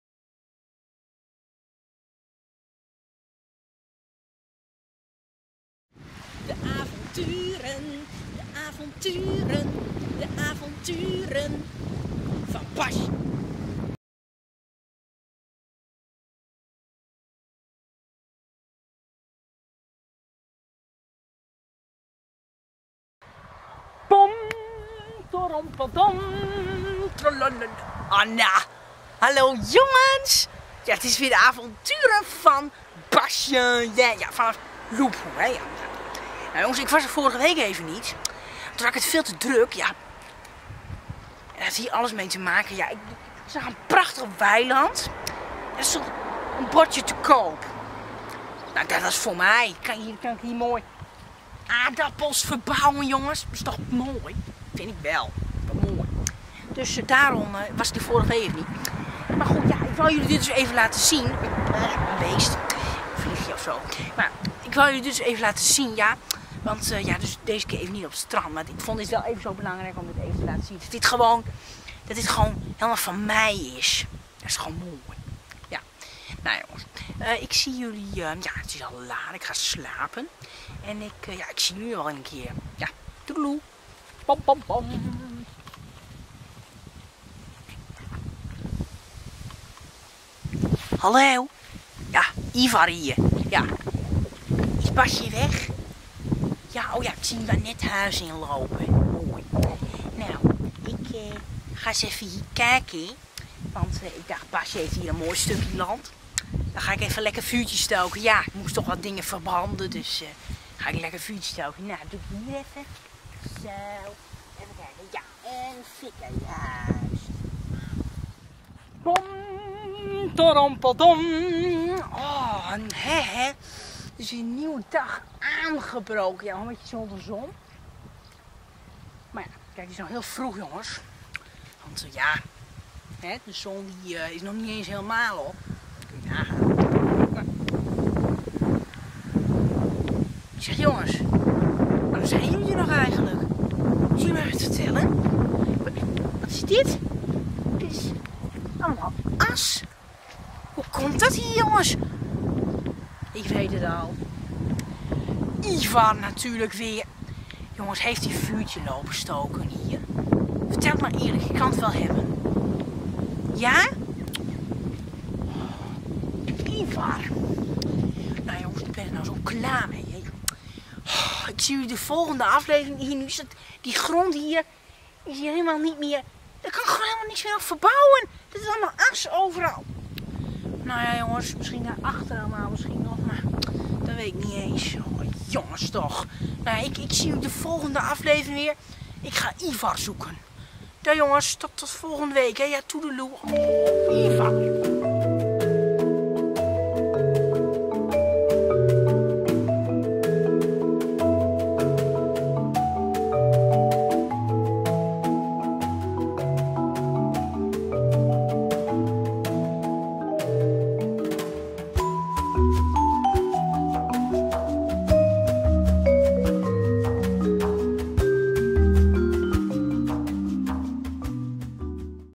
De avonturen van Basje vanaf Lupu - Aflevering 3, Seizoen 2: Basje's stuk grond
De derde aflevering van mijn zomerserie 'De avonturen van Basje vanaf Lupu'. Deze serie is een spin-off van mijn oude serie 'De avonturen van basje'.